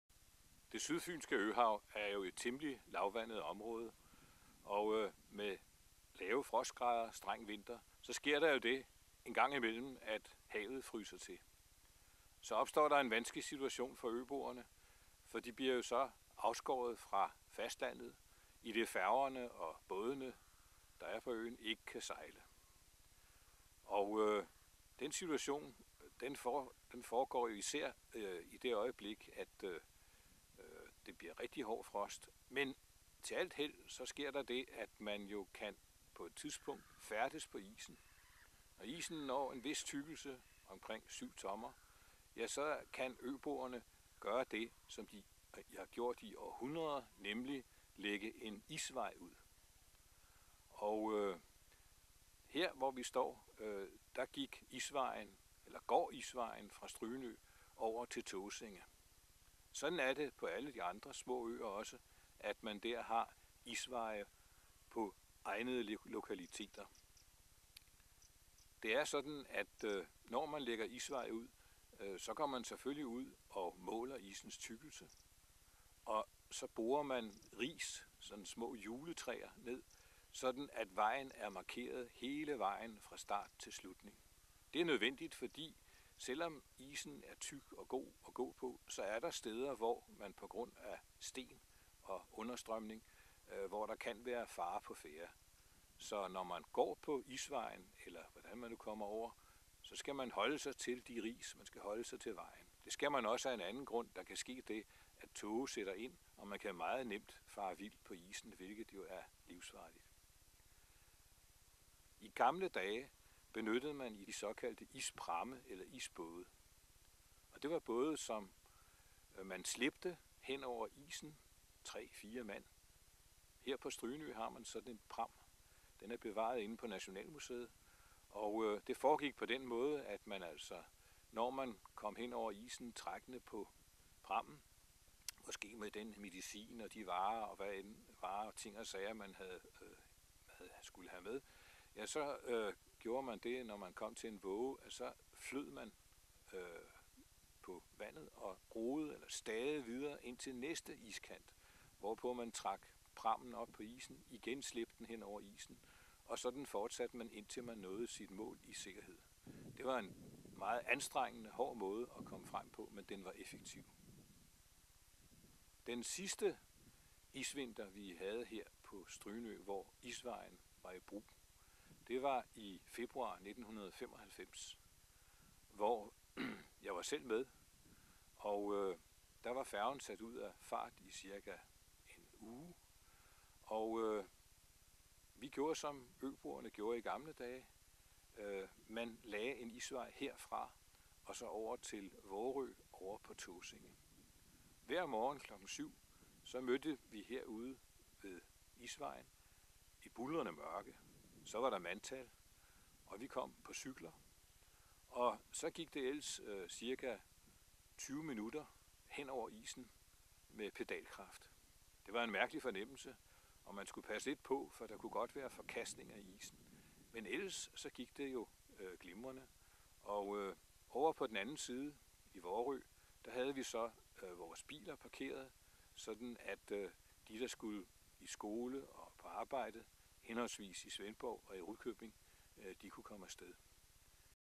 Isvejen fra Strynø
Historie om isveje i Det Sydfynske, hvor man om vinteren må bevæge sig over isen fra de små øer til fastlandet.